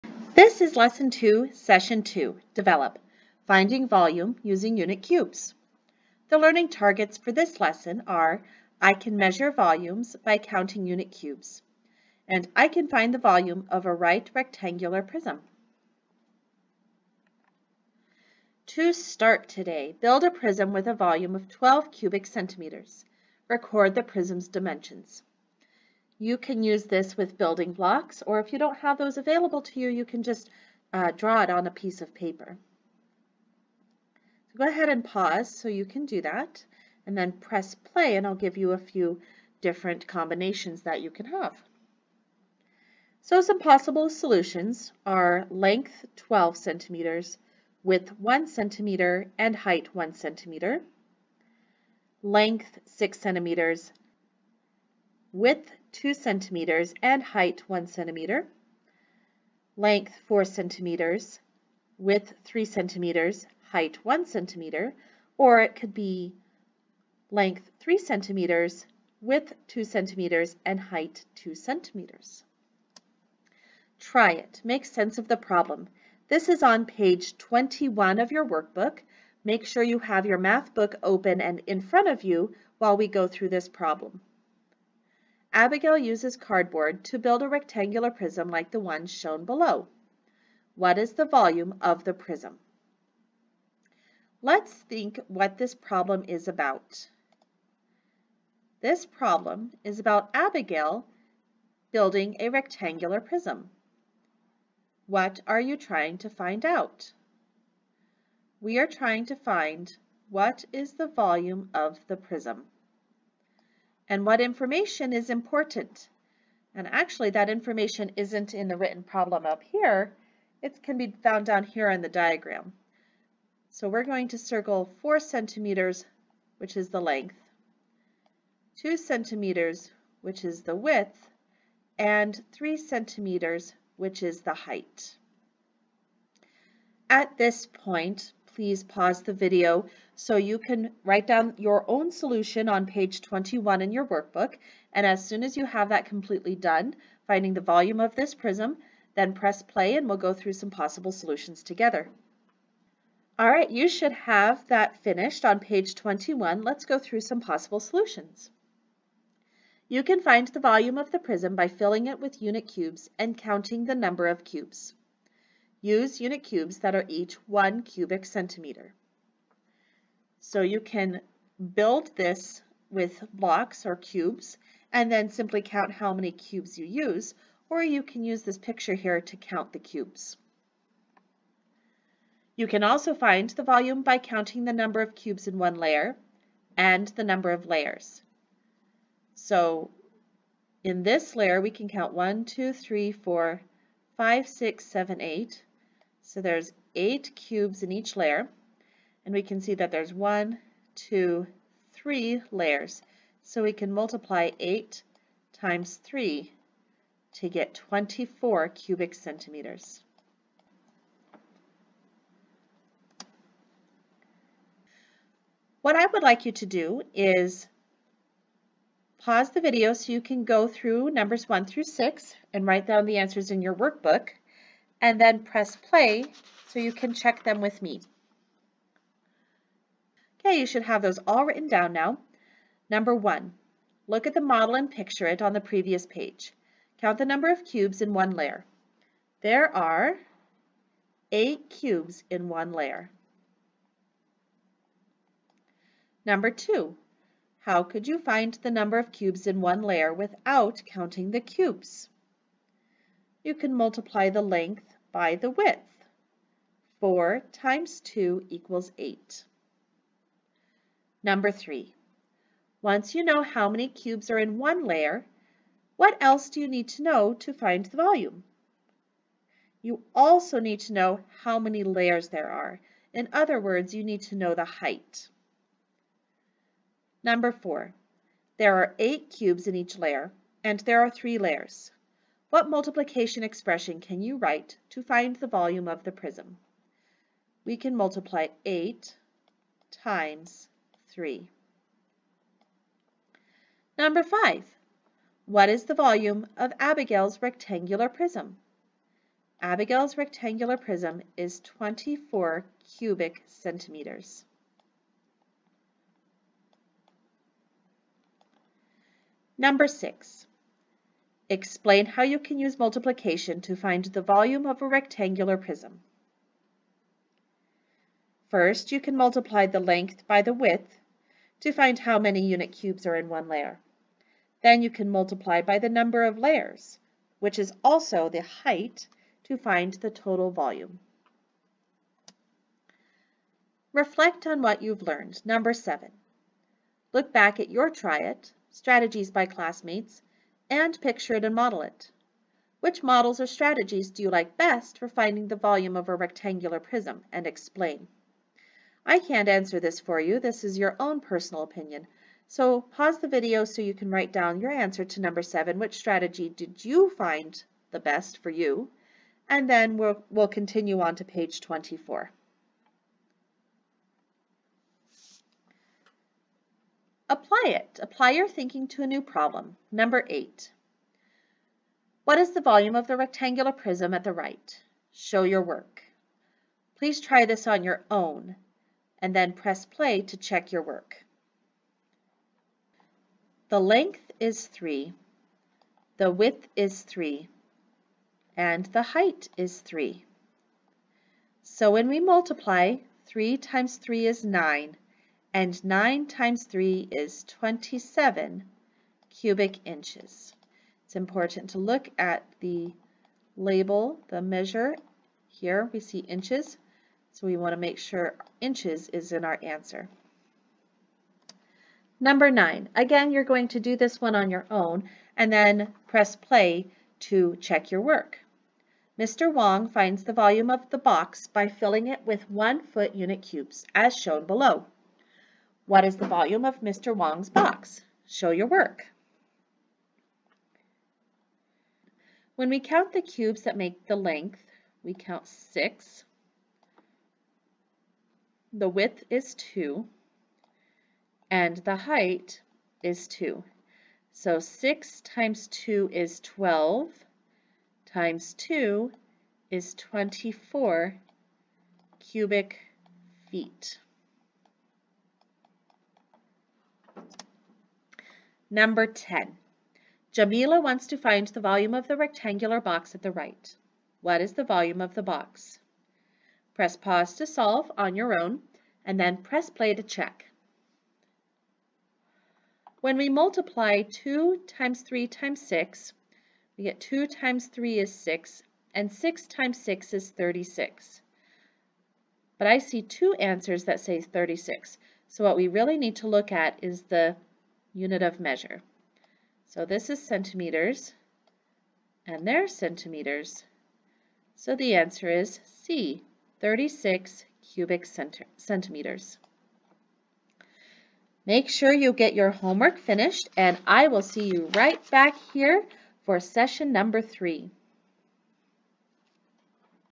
Math Lesson 2, Session 2 - Finding Volume Using Unit Cubes
Ready Classroom Math Grade 5 - Lesson 2, Session 2 - Finding Volume Using Unit Cubes, pg. 21